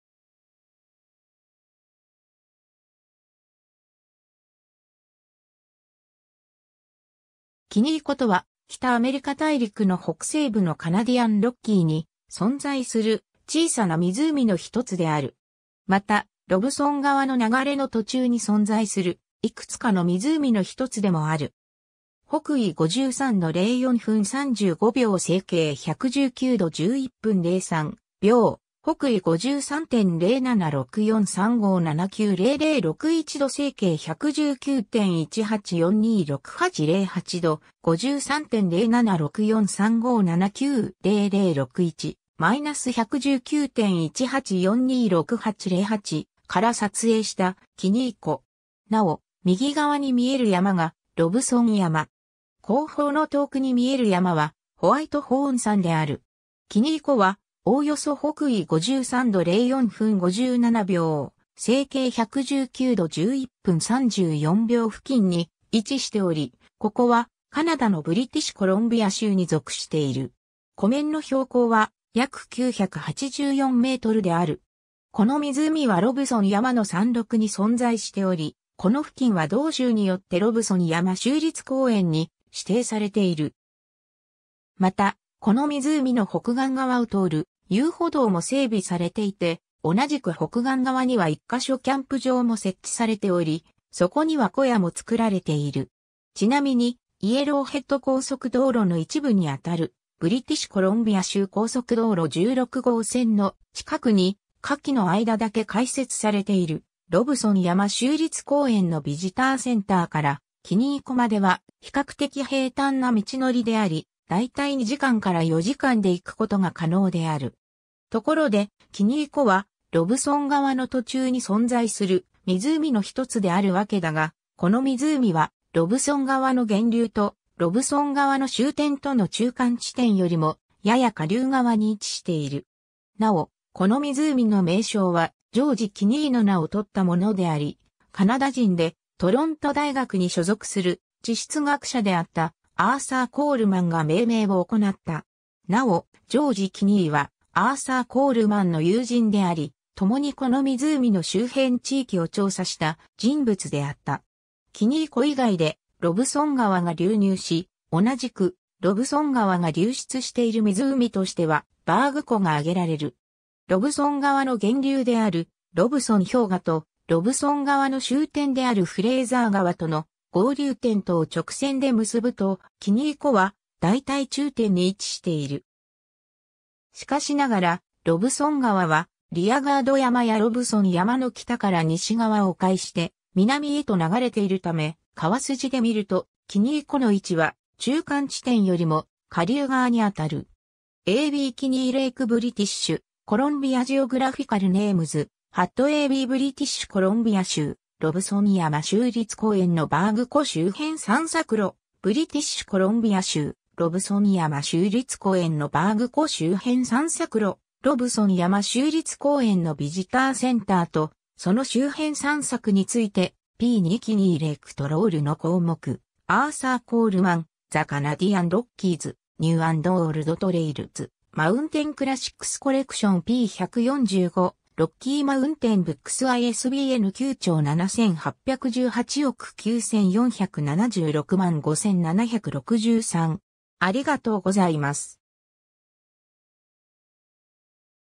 キニー湖
カナディアン・ロッキー
キニー湖（英語: Kinney Lake）とは、北アメリカ大陸の北西部のカナディアン・ロッキーに存在する、小さな湖の1つである。
また、ロブソン川の流れの途中に存在する、幾つかの湖の1つでもある。
 北緯53度04分35秒 西経119度11分03秒 / 北緯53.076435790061度 西経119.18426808度 / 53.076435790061; -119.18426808から撮影したキニー湖。
なお、右側に見える山がロブソン山。
後方の遠くに見える山は、ホワイトホーン山である。
 キニー湖は、おおよそ北緯53度04分57秒、西経119度11分34秒付近に位置しており、ここはカナダのブリティッシュコロンビア州に属している。
 湖面の標高は、約984mである。
 この湖はロブソン山の山麓（ロブソン山の山頂から見て南西の方向）に存在しており、この付近は同州によってロブソン山州立公園に指定されている。
また、この湖の北岸側を通る遊歩道も整備されていて、同じく北岸側には1箇所キャンプ場も設置されており、そこには小屋も作られている。
 ちなみに、イエローヘッド高速道路の一部に当たる、ブリティッシュコロンビア州高速道路16号線の近くに、夏季の間だけ開設されているロブソン山州立公園のビジターセンターからキニー湖までは、比較的平坦な道のりであり、だいたい2時間から4時間で行くことが可能である。
 ところで、キニー湖はロブソン川の途中に存在する（つまり、ロブソン川がこの湖に流れ込み、同じくロブソン川がこの湖から流れ出している）湖の1つであるわけだが、この湖は、ロブソン川の源流と、ロブソン川の終点（フレーザー川との合流点）との中間地点よりも、やや下流側に位置している。
 なお、この湖の名称は、ジョージ・キニー（George Kinney）の名を取ったものであり、カナダ人でトロント大学に所属する地質学者であったアーサー・コールマンが命名を行った。
 なお、ジョージ・キニーは、アーサー・コールマンの友人であり、共にこの湖の周辺地域を調査した人物であった。
 ^ キニー湖以外で、ロブソン川が流入し、同じくロブソン川が流出している湖としては、バーグ湖が挙げられる。
 ^ ロブソン川の源流であるロブソン氷河（氷河の先端にできているロブソン湖）と、ロブソン川の終点であるフレーザー川との合流点とを直線で結ぶと、キニー湖はだいたい中点に位置している。
しかしながら、ロブソン川はリアガード山やロブソン山の北から西側を迂回して南へと流れているため、川筋で見ると、キニー湖の位置は中間地点よりも下流側に当たる。
 ^ a b Kinney Lake British Columbia Geographical Names （ブリティッシュコロンビア州の地名） ^ a b ブリティッシュコロンビア州、ロブソン山州立公園のバーグ湖周辺散策路 （ここの右下に書かれている小さな図を参照。） ^ ブリティッシュコロンビア州、ロブソン山州立公園のバーグ湖周辺散策路 ^ ロブソン山州立公園のVisitor Centreと、その周辺散策について p.2 （右側下部） Kinney Lake Trallの項目 ^ アーサー・コールマン（Arthur Philemon Coleman） 『The Canadian Rockies ～ New and Old Trails ～ Mountain Classics Collection』 p.145 Rocky Mountain Books ISBN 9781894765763